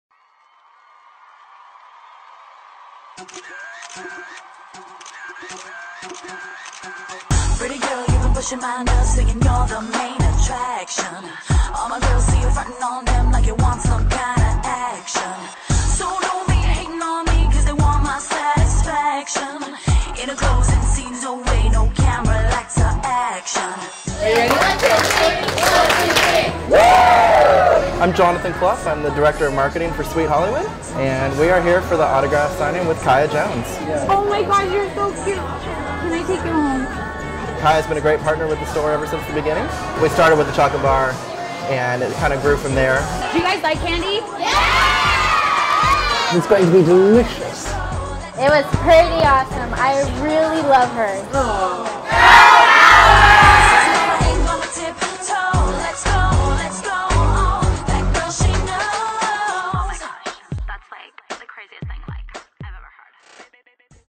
KAYA JONES - The Hollywood Doll WITH FANS AT SWEET!
KAYA - The Hollywood Doll spending time with some future Hollywood Dolls at The official Hollywood Doll pop up shop at SWEET!